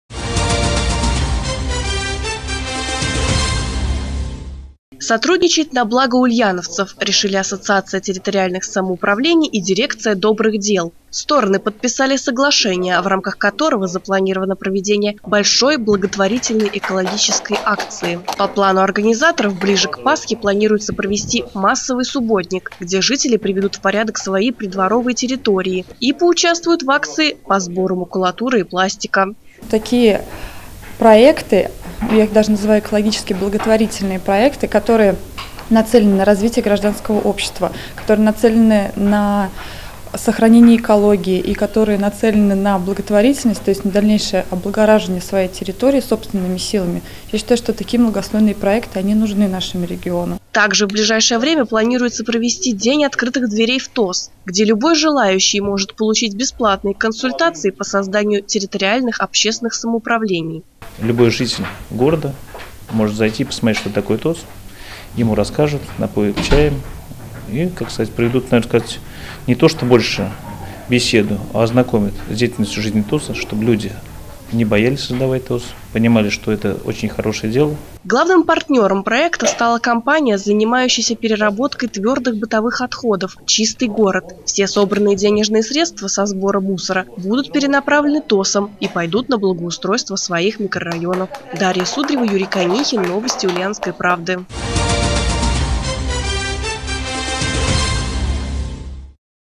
На благо жителей
Ульяновские благотворители и ТОСы подписали соглашение, в рамках которого планируется проведение субботников и работ по благоустройству.